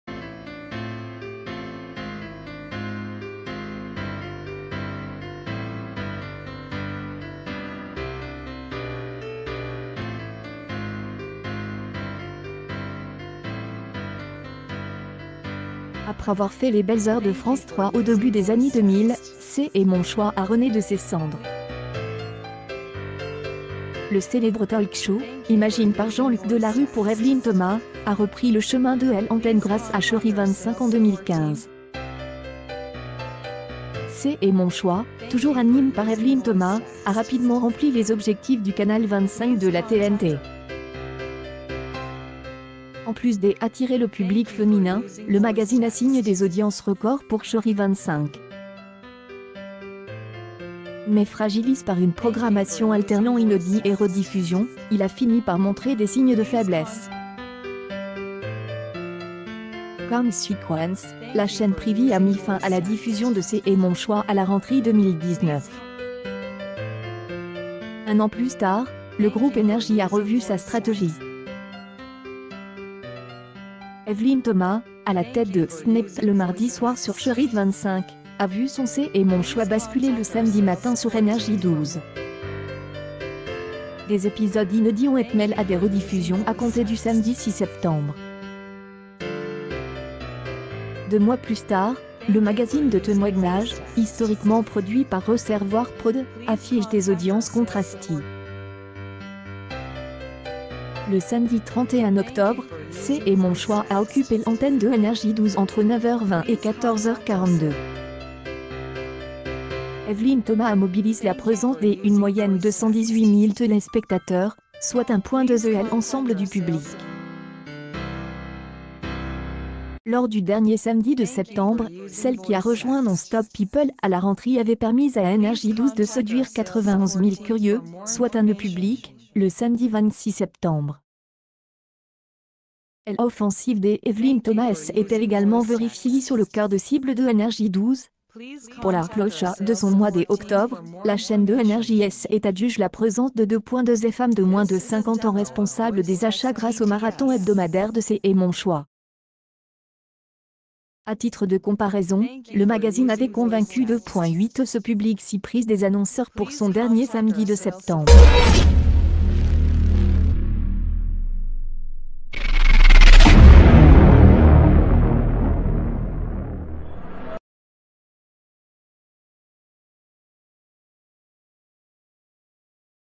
C’est mon choix : Evelyne Thomas change de chaîne, NRJ12 gagnante en audience ? |toutelatele365
,#marabout,#stand-up,#bestof,#de,#retour
C’est mon choix : Evelyne Thomas change de chaîne, NRJ12 gagnante en audience ? |toutelatele365 Après avoir fait les belles heures de France 3 au début des années 2000, C’est mon choix a renaît d